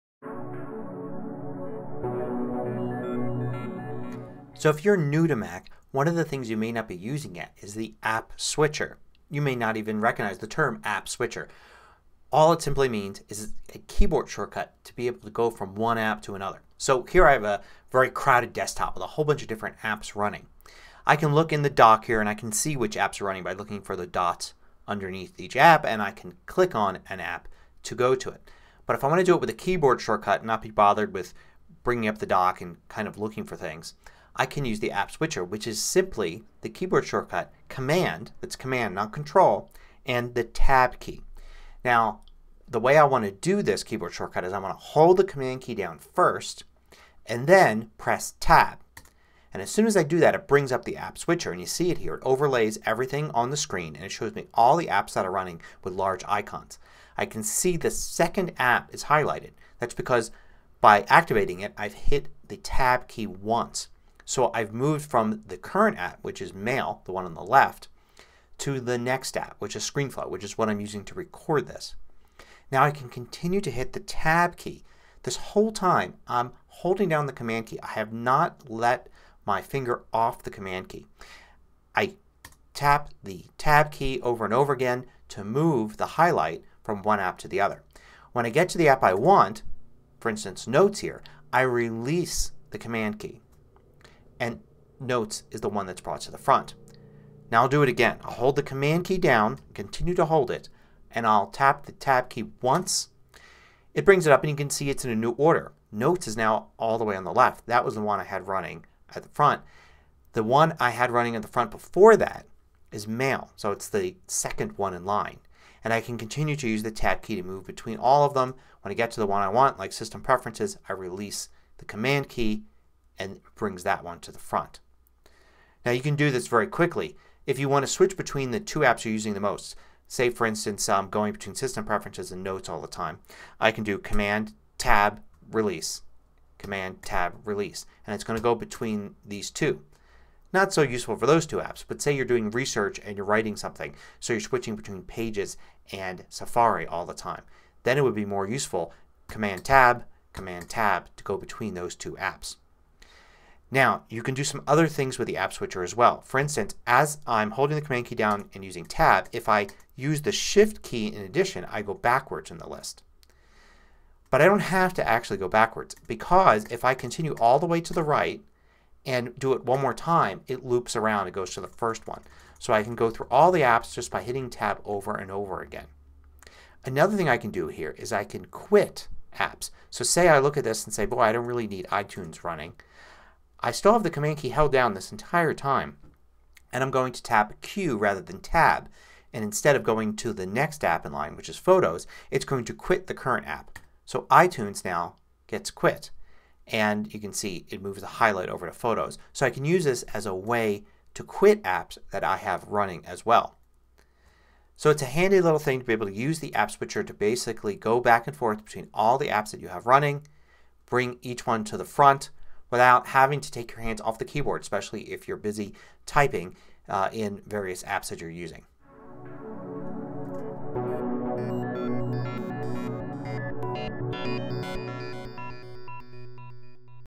A Beginner's Guide To the Mac App Switcher (#1543)
You can use the App Switcher to quickly and easily switch between running apps. It is a keyboard shortcut, but not as simple to use as just pressing the keys. You can use it to see which apps are running and pick which one to bring to the front, all without lifting your fingers off of the keyboard. You can also use it to quit apps.